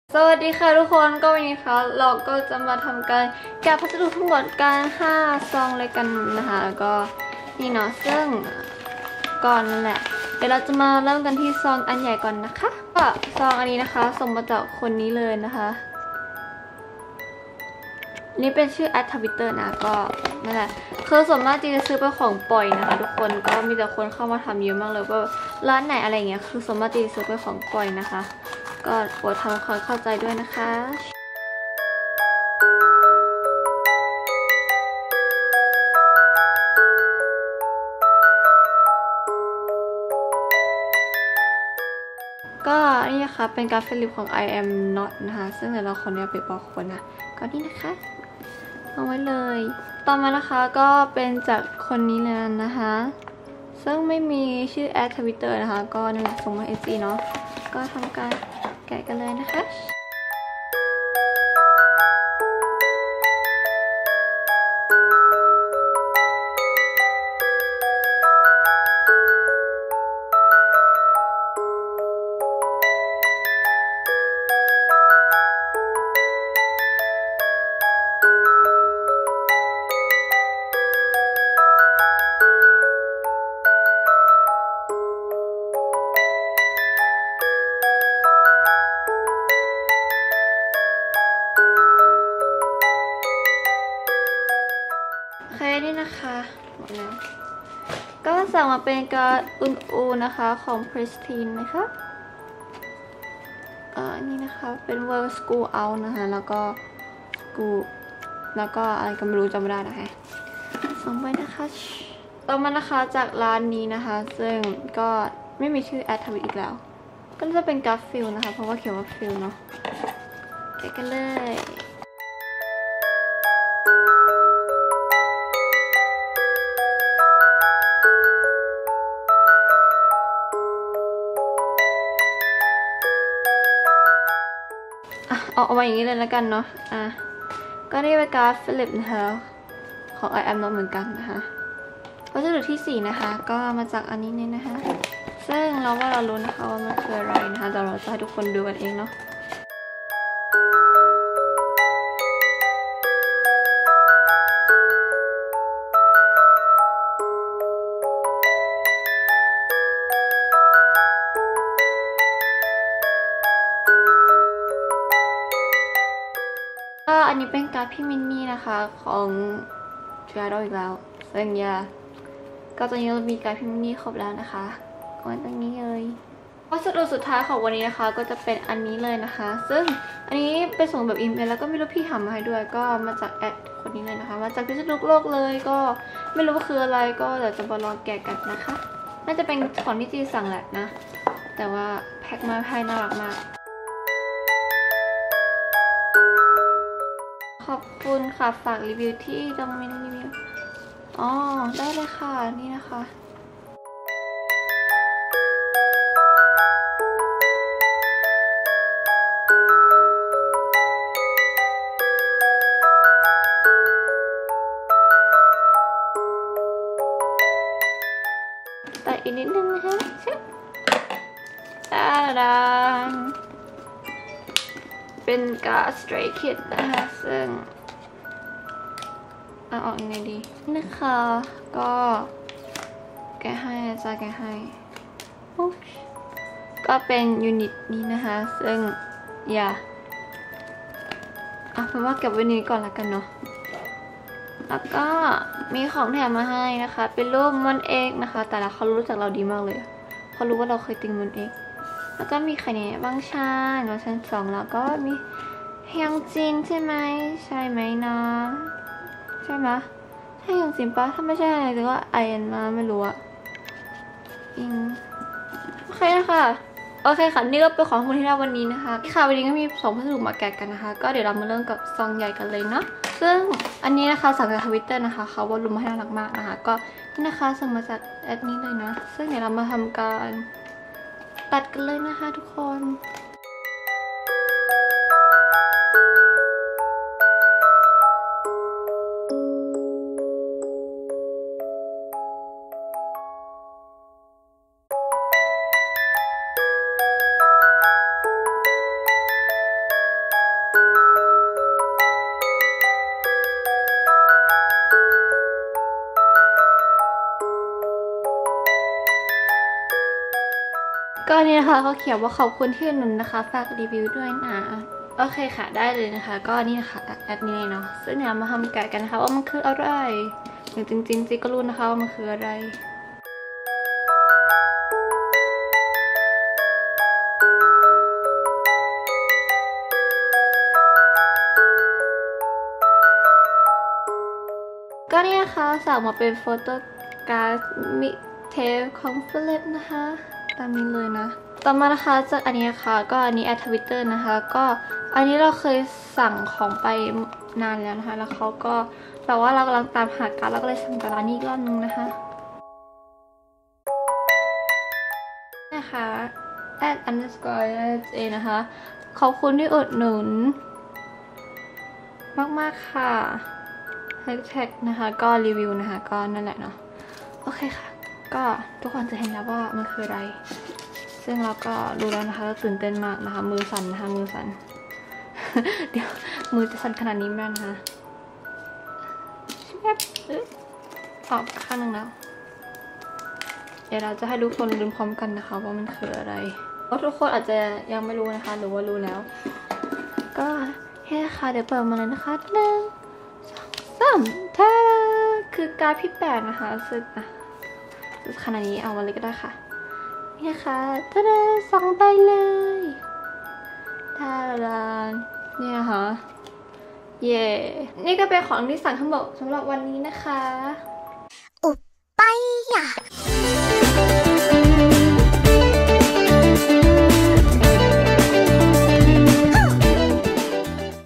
[ HAUL ] มาอีกแล้วเรอะ? | GinaJJ 💕
อันยองค่าาา วันนี้มาทำคลิปแกะการ์ดอีกแล้ว จีรู้ว่าทุกคนเบื่อ😂 แต่เอาไว้ดูแก้เหงานะ เพลินๆ😝

เดี๋ยวเรามาทำความรู้จักกับจีกันดีกว่า🖤

ชื่อเล่น : จีน่า  / Gina / 지나
เกิดปี : 2545 / 2002
อยู่ : ประเทศไทย / THAILAND 🇹🇭

สามารถเม้นคุยกันได้ค่า😁